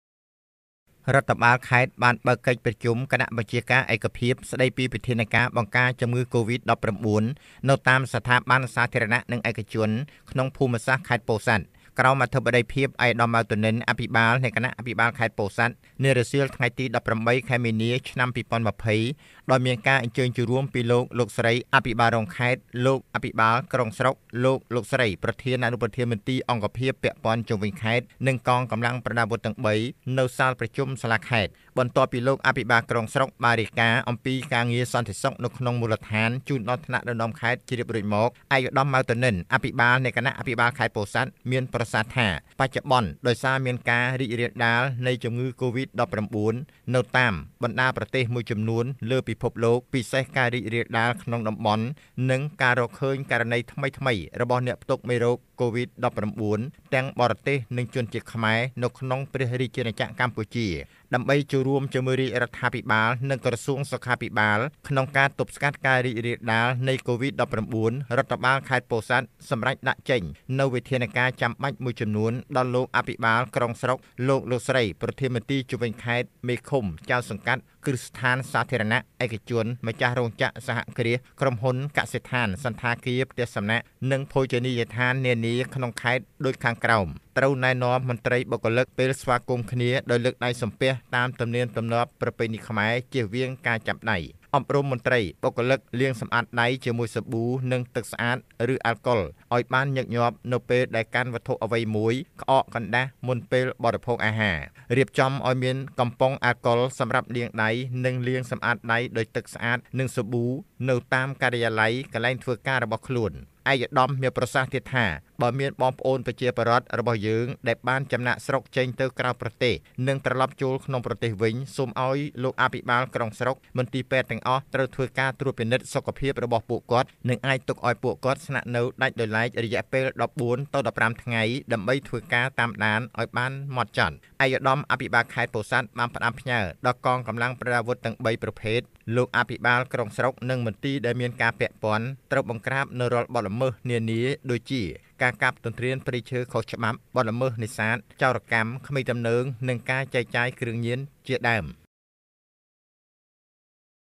កិច្ចប្រជុំគណៈបញ្ជាការឯកភាព ស្តីពីវិធានការបង្កាជំងឺកូវីដ១៩ នៅតាមស្ថាប័នសាធារណៈ
កិច្ចប្រជុំគណៈបញ្ជាការឯកភាព ស្តីពីវិធានការបង្កាជំងឺកូវីដ១៩ នៅតាមស្ថាប័នសាធារណៈ និងឯកជនក្នុងភូមិសាស្រ្តខេត្តពោធិ៍សាត់ ក្រោមអធិបតីភាព ឯកឧត្តម ម៉ៅ ធនិន អភិបាលនៃគណៈអភិបាលខេត្តពោធិ៍សាត់ នារសៀលថ្ងៃទី១៨ ខែមីនា ឆ្នាំ២០២០ ដោយមានការអញ្ជើញចូលរួមពីលោក លោកស្រីអភិបាលរងខេត្ត លោក អភិបាលក្រុង ស្រុក លោក លោកស្រី ប្រធាន អនុប្រធាន និងកងកម្លាំងអាវុធទាំងបី នៅសាលប្រជុំសាលាខេត្ត។